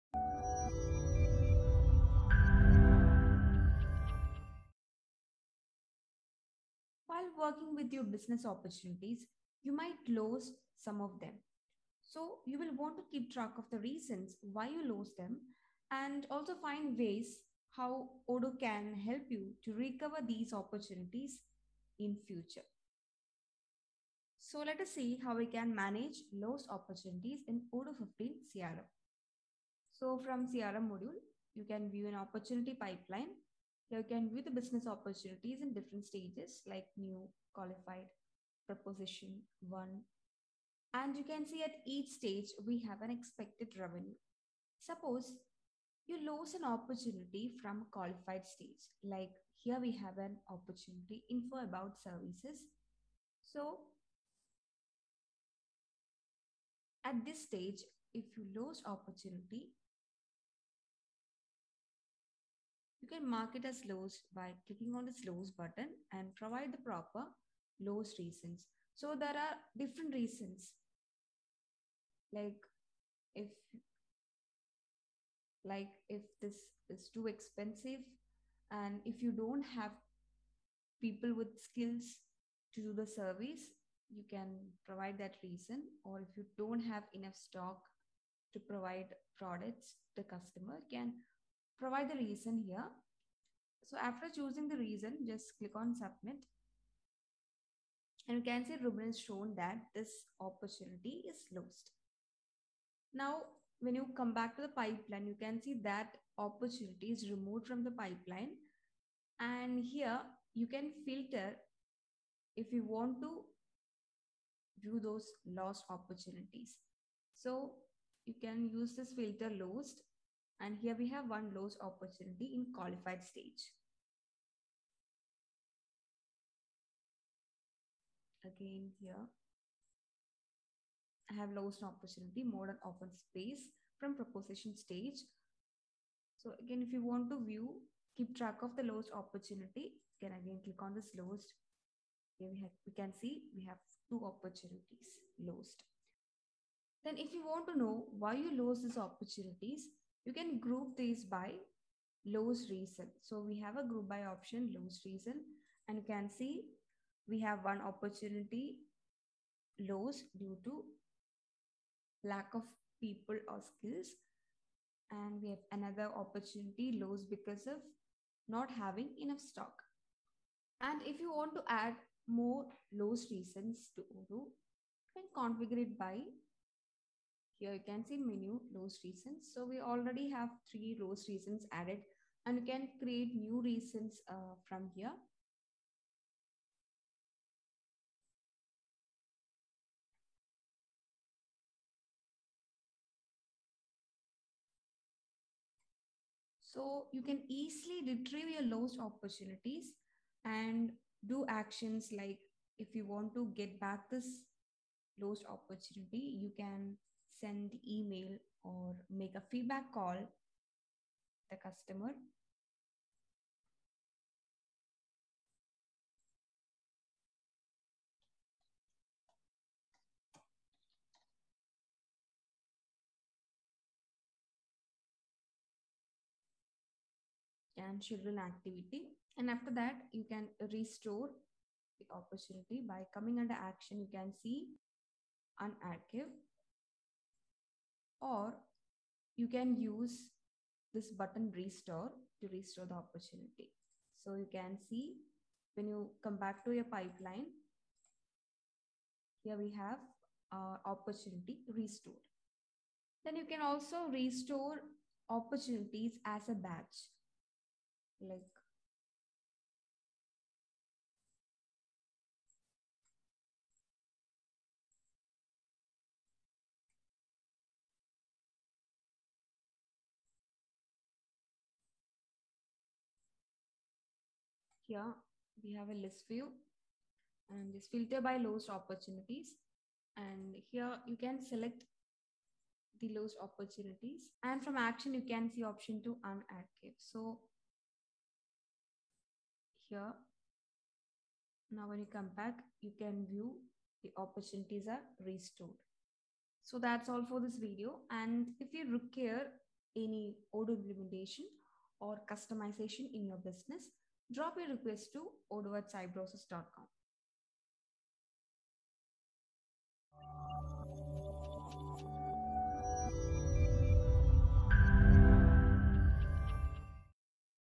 Managing Your Lost Opportunities in Odoo 15 CRM | Odoo 15 Enterprise Edition
While working with your opportunities, you might lose some of them. You will want to keep track of the reasons you lost them and also which ways Odoo can help you recover them in the future.

While in your pipeline, select any opportunity you want and you will see a Lost button.

You can then select an existing Lost Reason or create a new one right there.